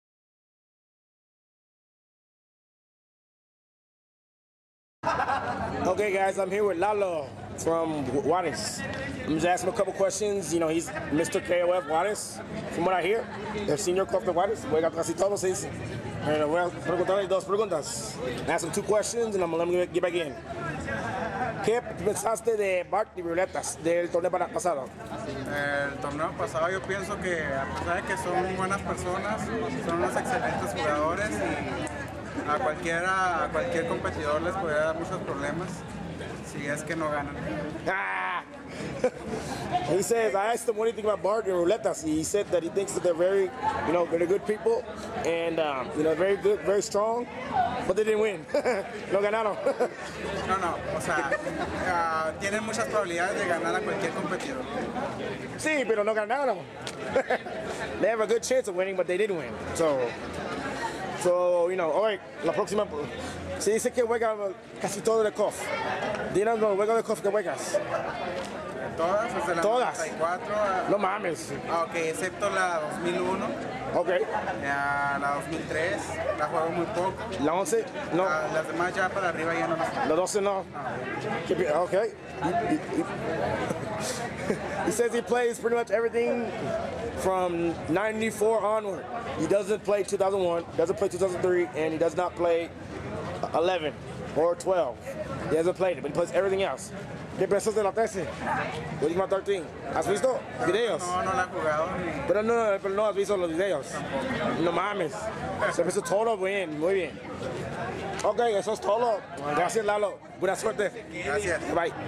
Battle 4 Mexico 2010 Lalo from Ciudad Juarez, Mexico Interview
Interview with Lalo from Juarez, Mexico who is silently making a name for himself by continuing to rank top 3/4 in big tournaments in Juarez since JuareZ WarZone. He is the brother of Esteban yes.